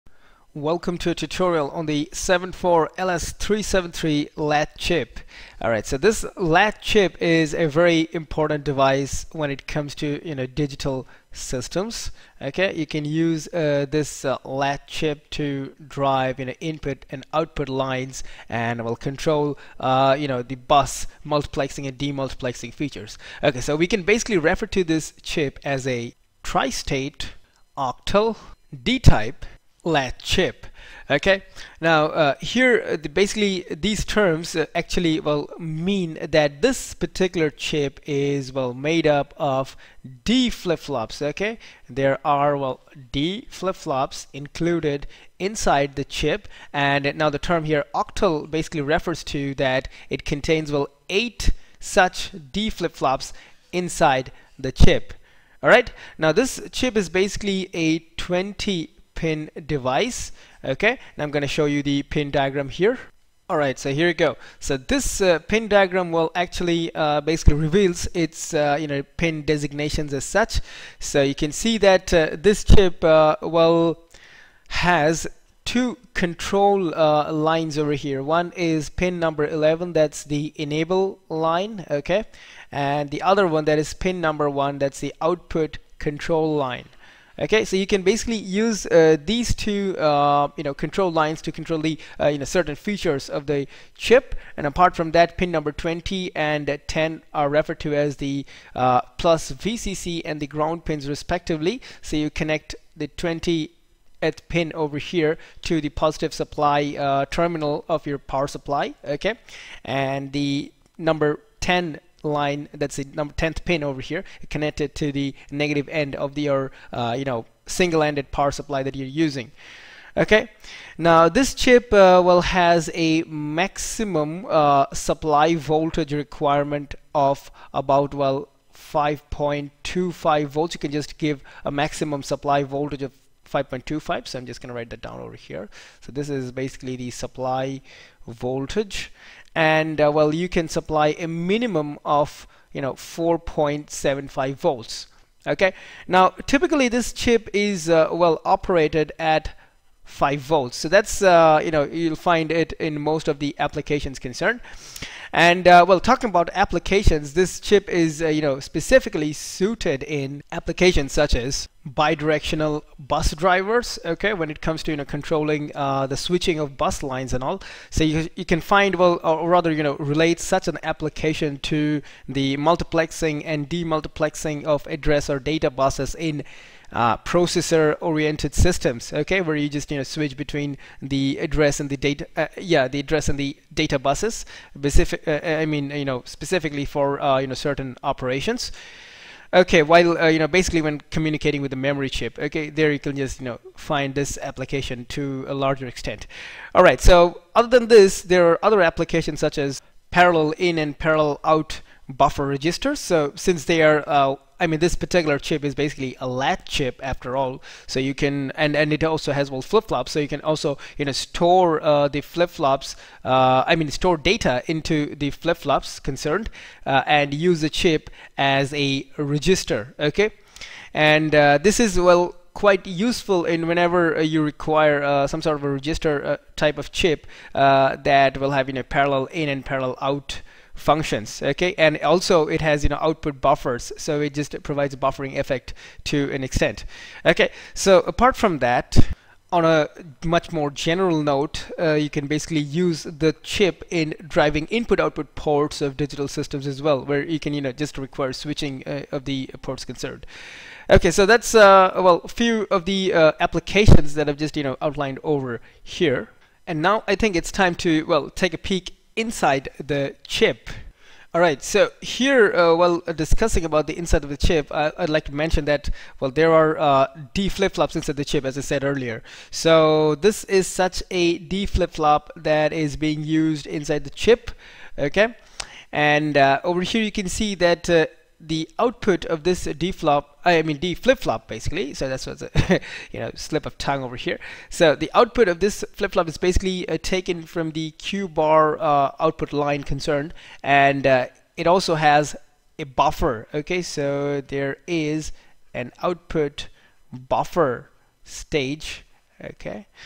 74LS373 LATCH CHIP || Microprocessor
74LS373 LATCH is the 7th video tutorial within “8085 Essentials” module of Microprocessor Course. This tutorial aims to explain Concepts of Tristate, Buffers & Fan Out. Pin description and Internal block diagram of 74373 has been discussed in great detail. Truth table & its applications has also been an integral part of this tutorial.